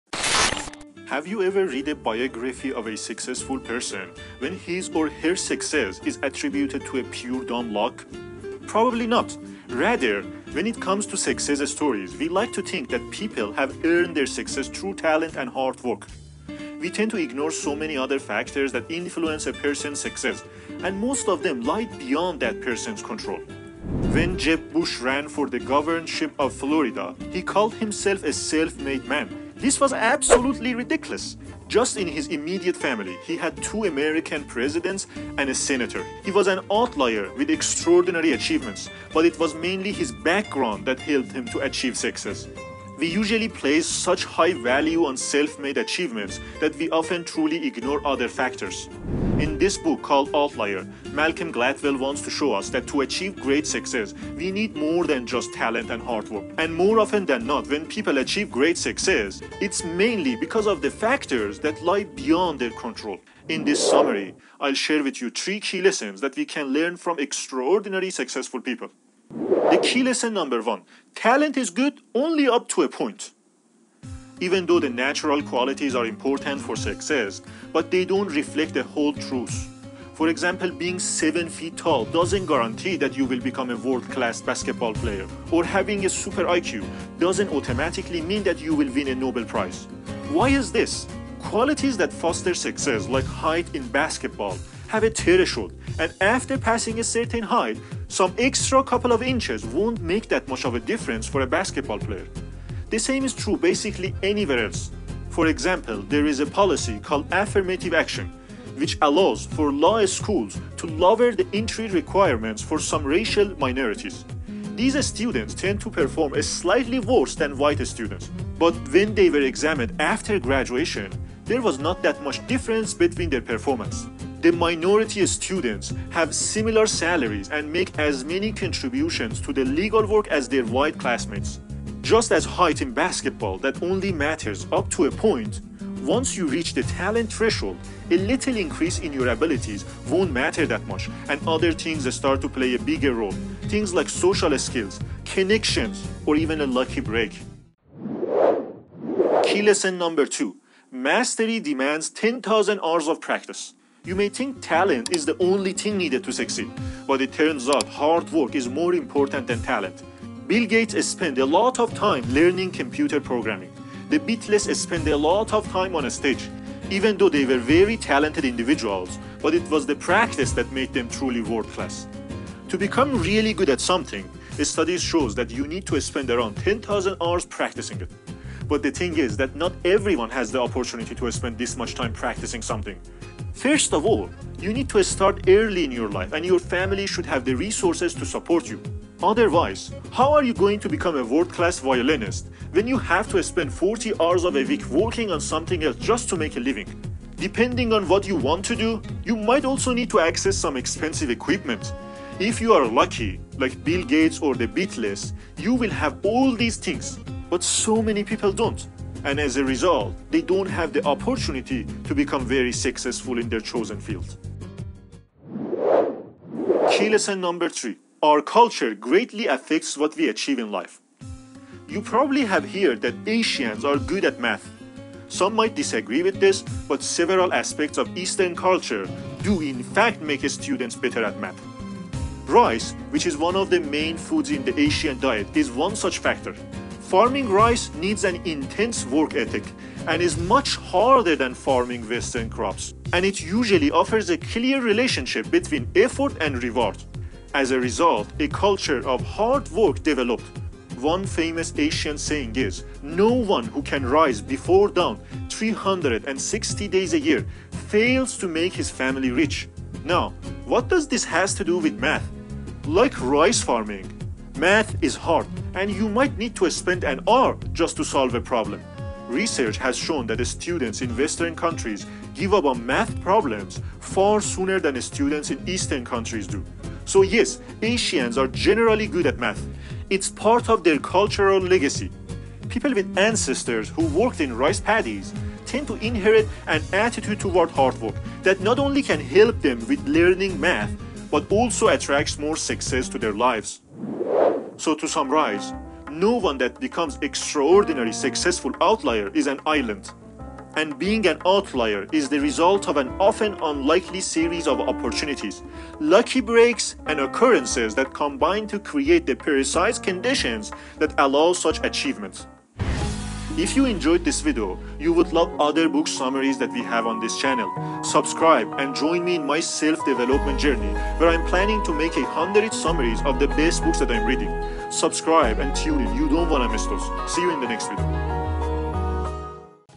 Outliers by Malcolm Gladwell | Animated Book Summary
This is the animated book summary of The Outliers by Malcolm Gladwell. In this video, I'll share with you three lessons that we can learn from extraordinary successful people.

This book on Amazon

★★★ TIMESTAMPS ★★★

0:00 Intro
1:08 The key lesson #1: Talent is good only up to a point
2:25 The key lesson #2: Mastery demands 10,000 hours of practice
3:35 Key lesson #3: Our culture greatly affects what we achieve in life.
4:54 Summary